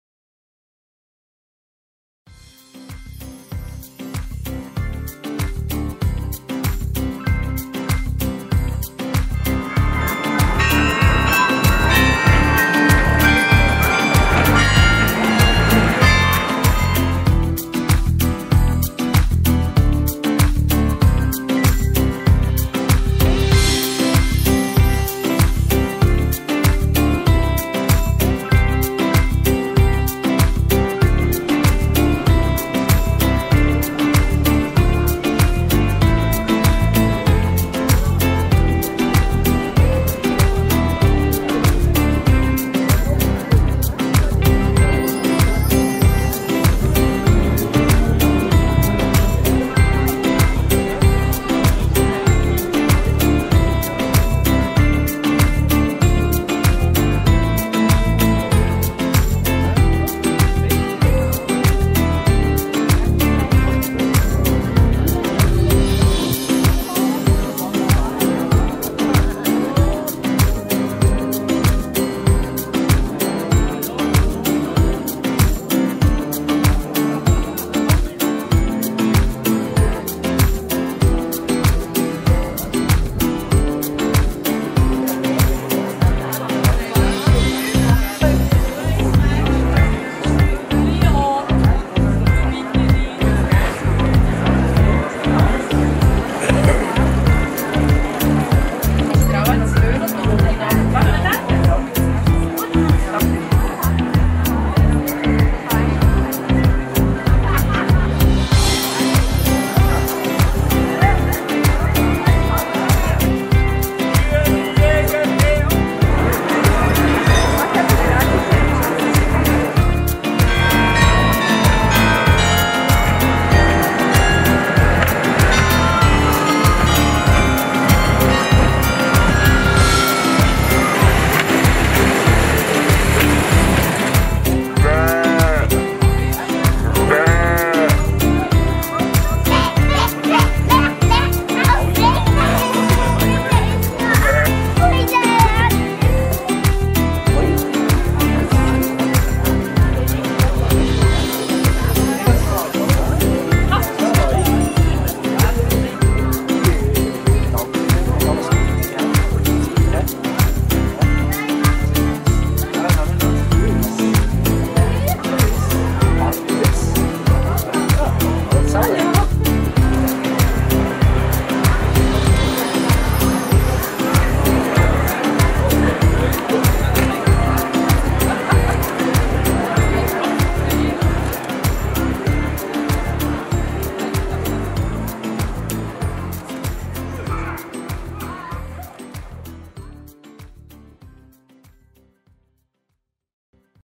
Christkindli Märt Willisau 2018
Besuch Weihnachtsmarkt 2018 im idyllischen Stätchen Willisau/LU.